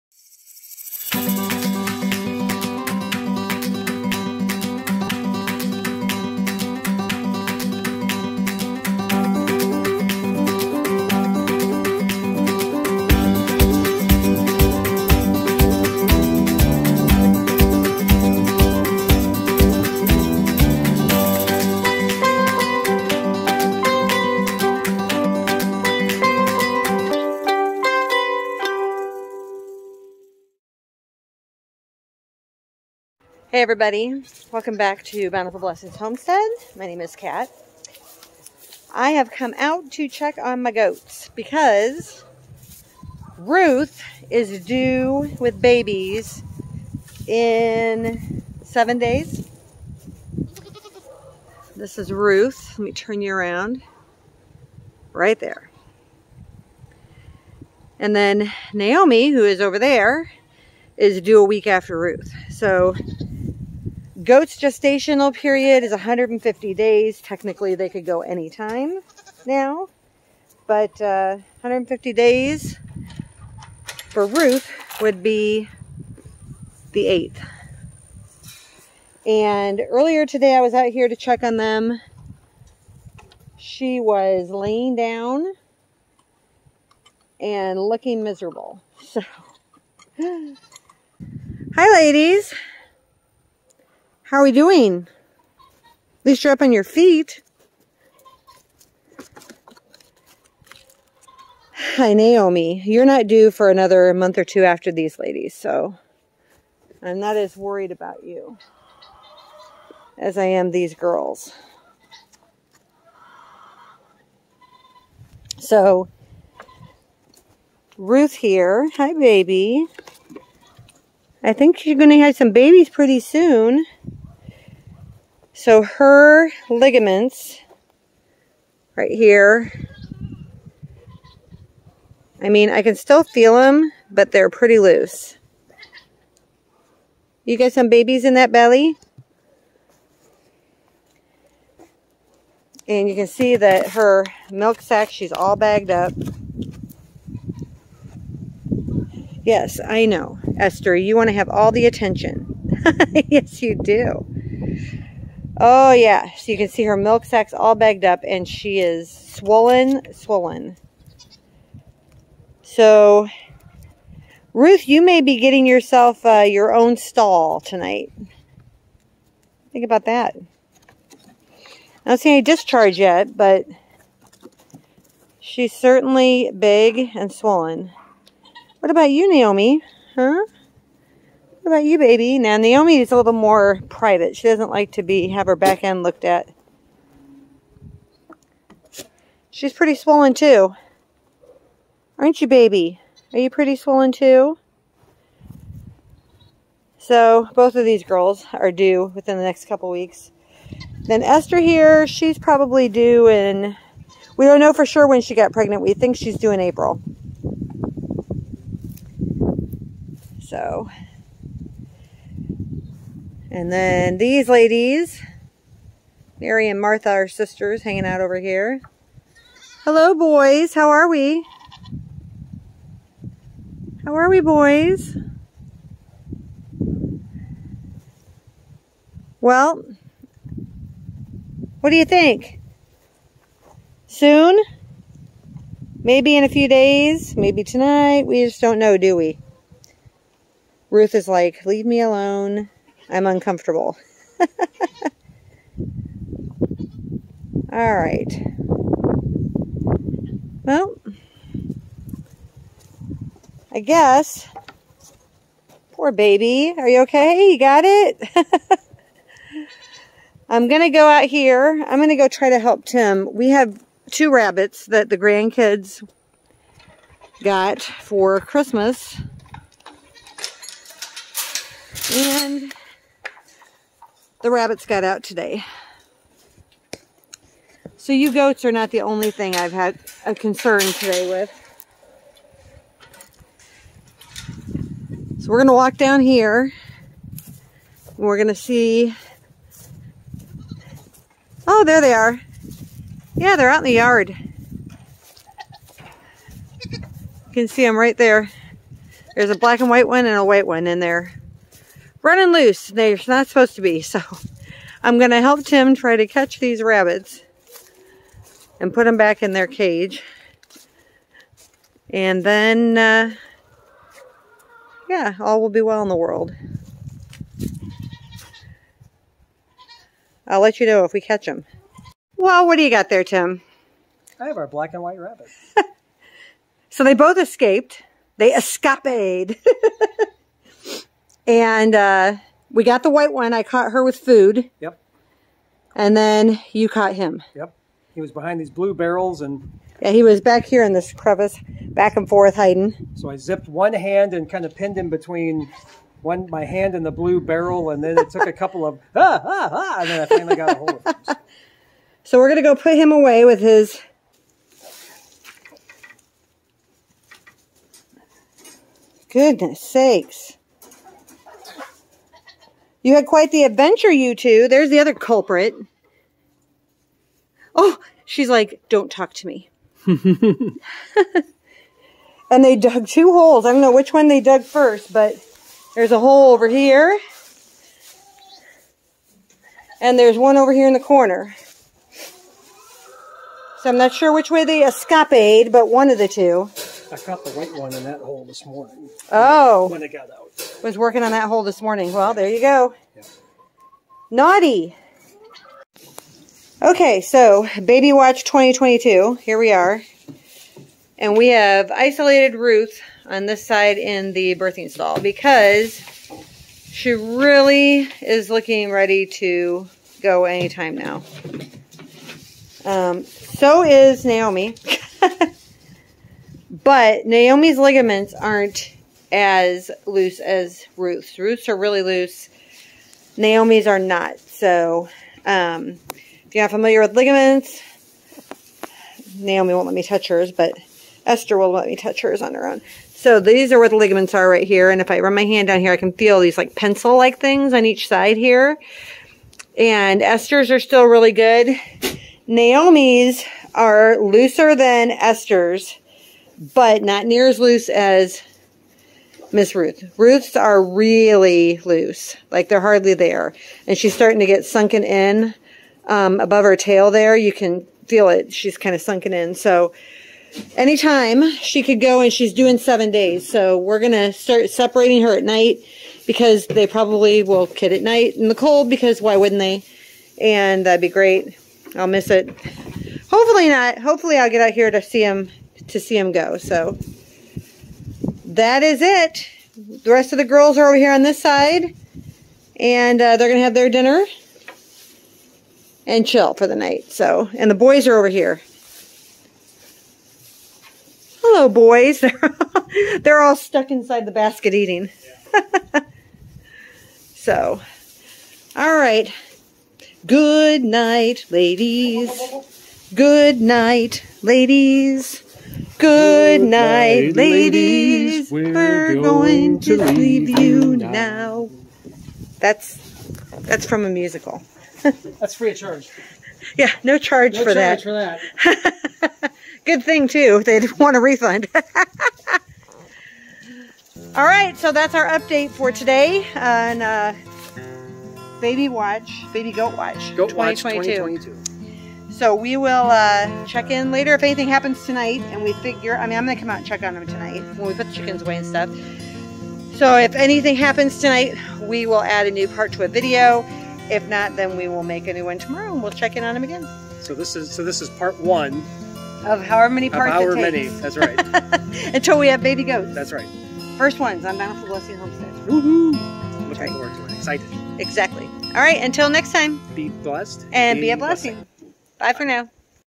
Baby Goat Watch 2022 (and the rabbits got out!)
It's official!  We are less than a week out from Ruth's due date and her sister Naomi is due a week after her!  Both girls look so uncomfortable and are displaying signs of getting close to delivery!  Follow along with us as we prepare for BABY GOATS!!!